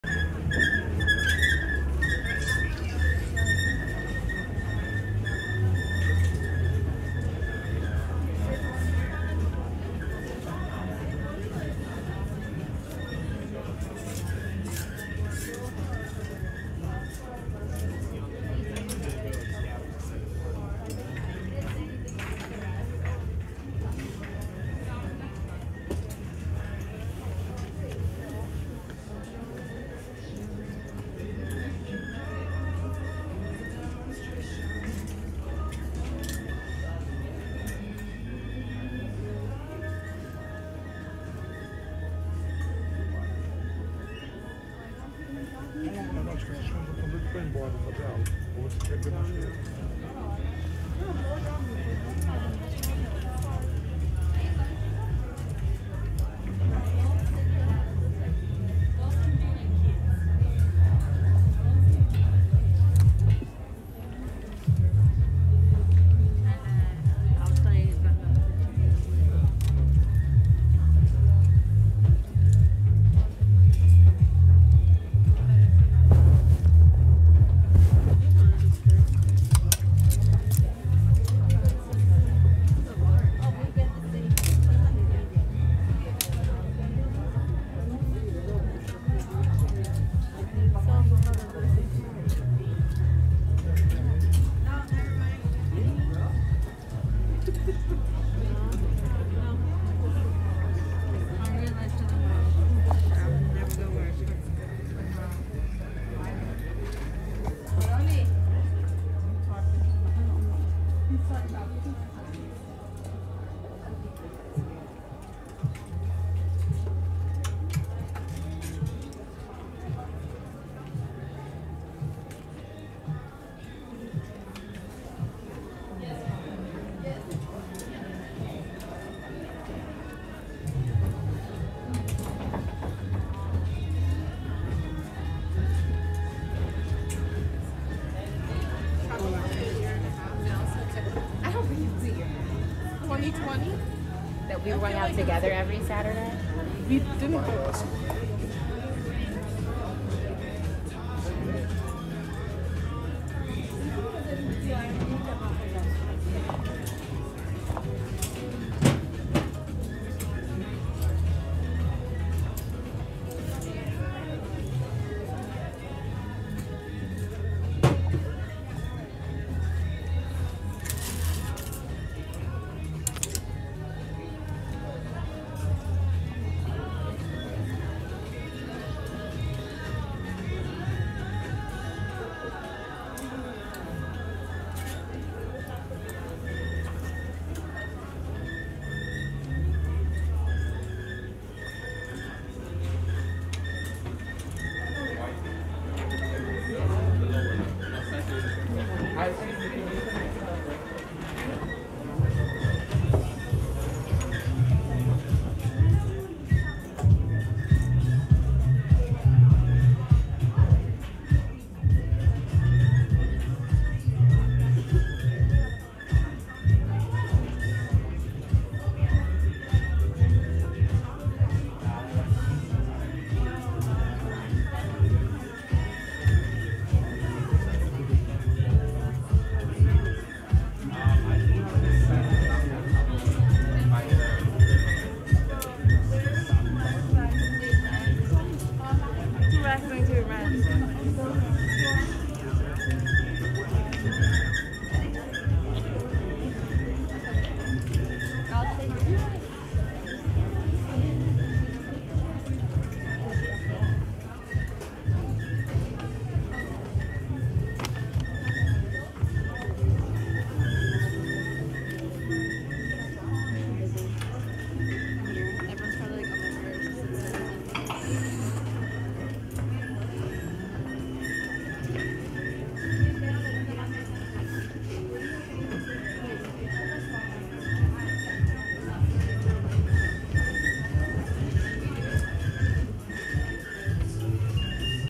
Walk with me at Zara store , Manhattan, 90% off??
Walk with me at Zara store in Manhattan.
This location was pretty full and sale was running after holidays.
Deals were great and absolutely you can’t leave empty handed.